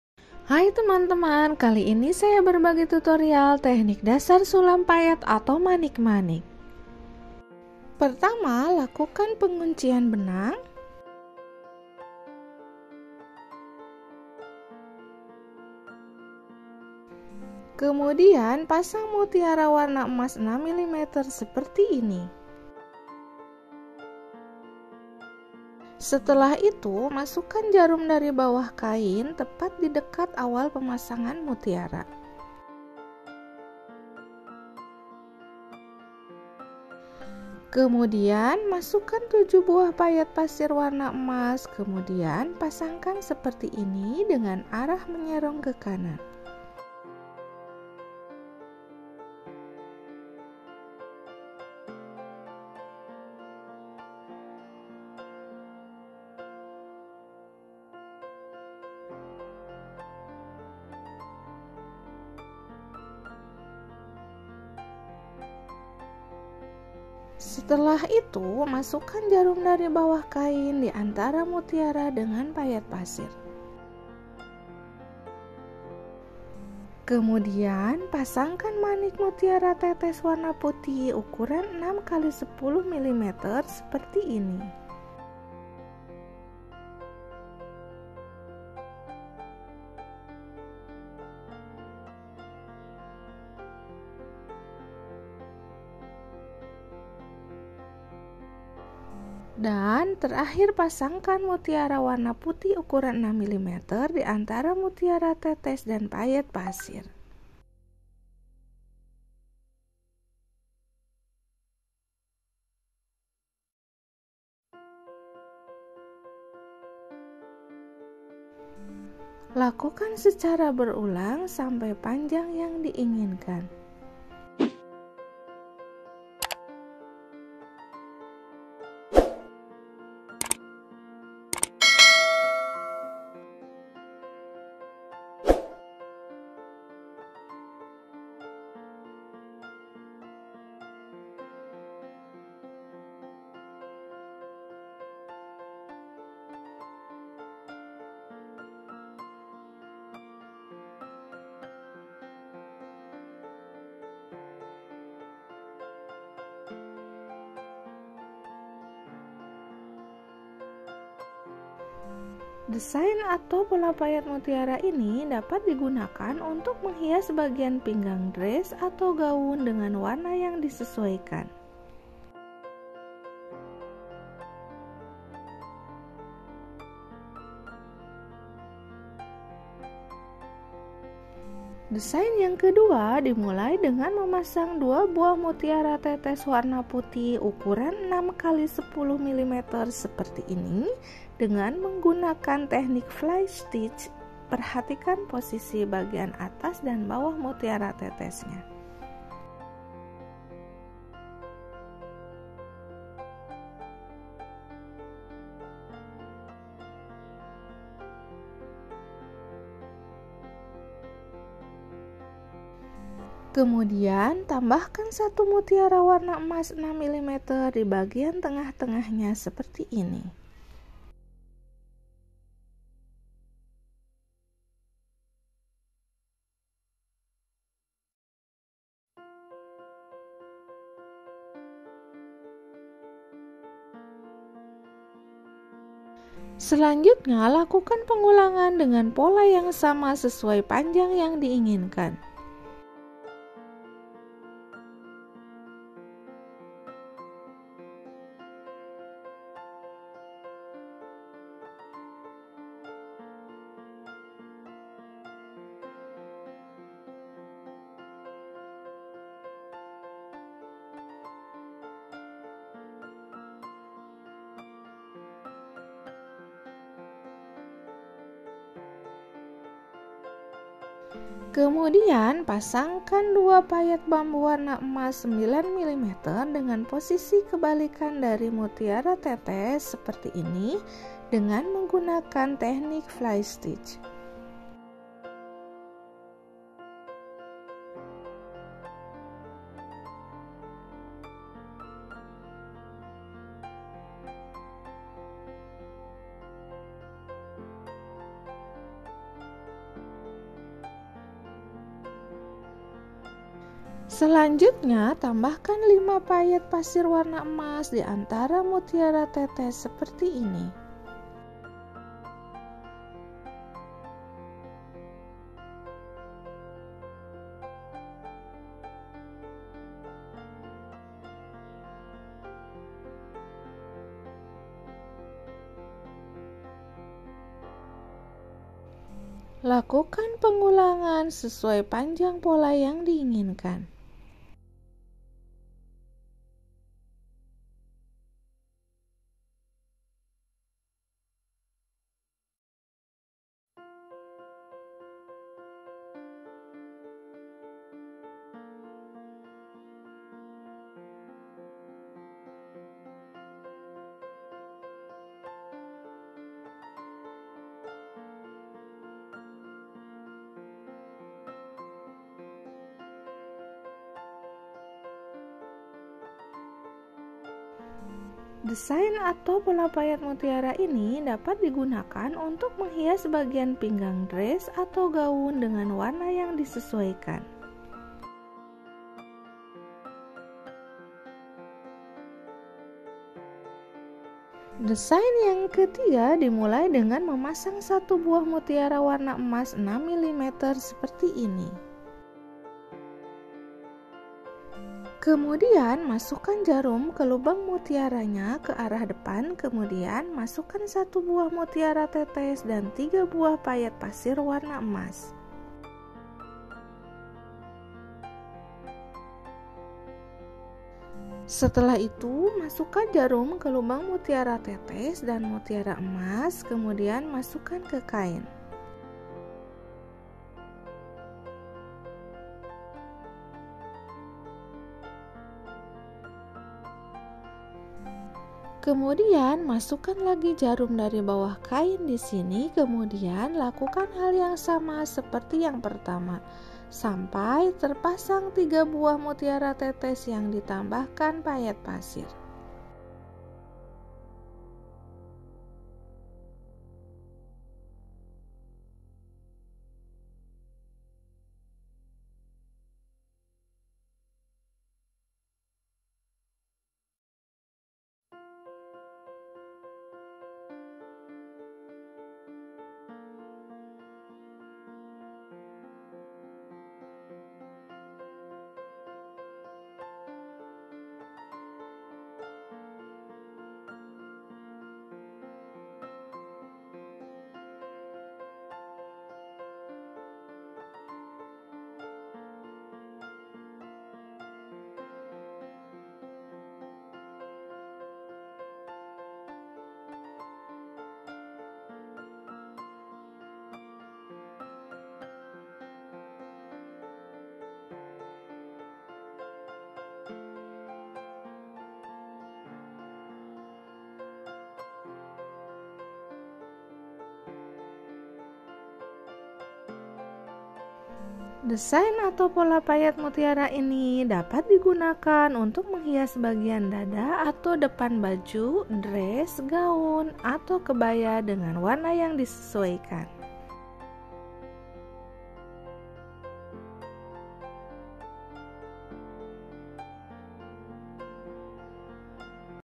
Beads Hand embroidery basic stitches tutorial for beginners || Beads embroidery design || Beads work
Tutorial teknik dasar sulam manik-manik  mutiara atau payet ini ini diperuntukan untuk pemula yang belajar sulam payet atau manik-manik. Disini saya menyajikan 3 teknik dasar sulam dan tiga desain manik-manik atau payet yang dapat diaplikasikan pada pakaian. Tutorial ini disajikan langkah demi langkahnya sehingga memudahkan pemula yang sedang belajar sulam manik-manik mutiara atau payet mutiara ini.

Alat dan bahan :
Hoop
Kain belacu
benang sulam/benang jahit
jarum payet
gunting
needle threader thread/mata nenek
manik-manik mutiara tetes 6 x 10 mm
manik-manik mutiara sintetis 6 mm
manik-manik pasir
manik-manik bambu 9 mm